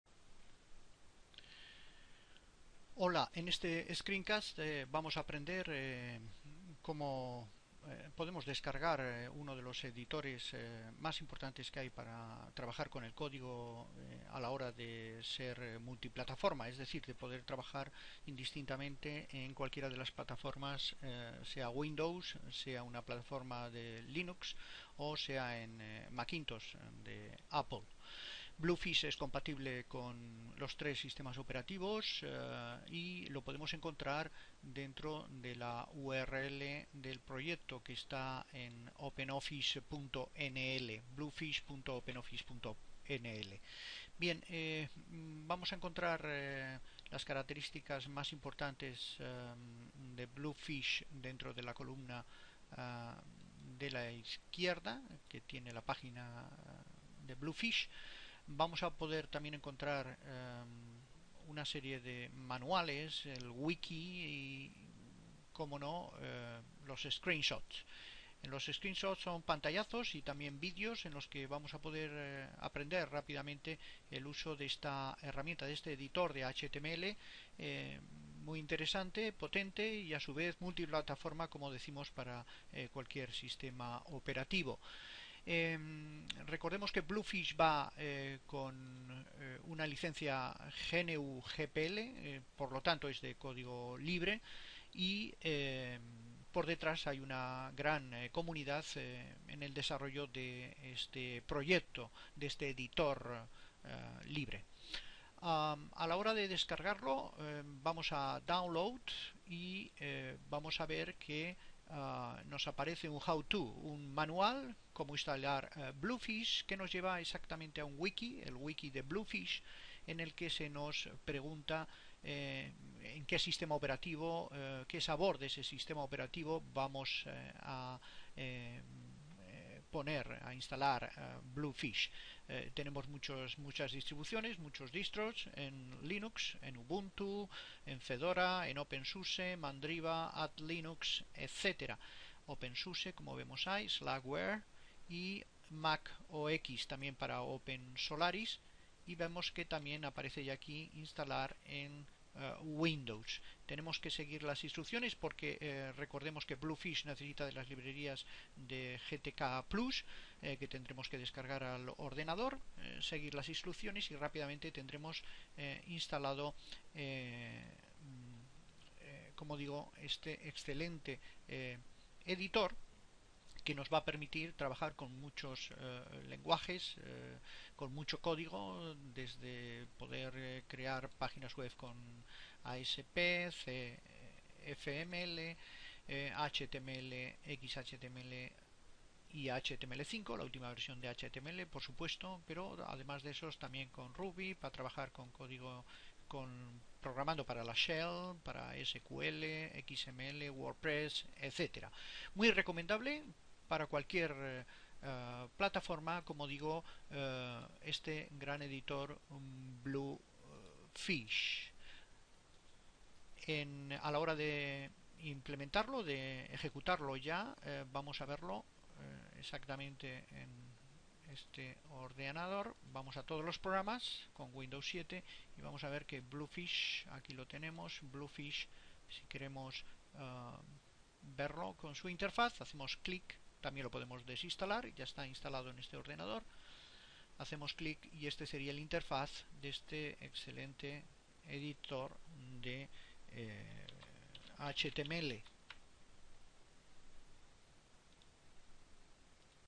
Editor multiplataforma Bluefish
Curso de HTML5.
El editor Bluefish puede ser instalado en cualquier plataforma: GNU/Linux (Mint, Ubuntu, Debian, Opensuse...), Windows (Vista, 7..) o Apple. Leed bien las instrucciones antes de descargarlo a vuestro sistema operativo.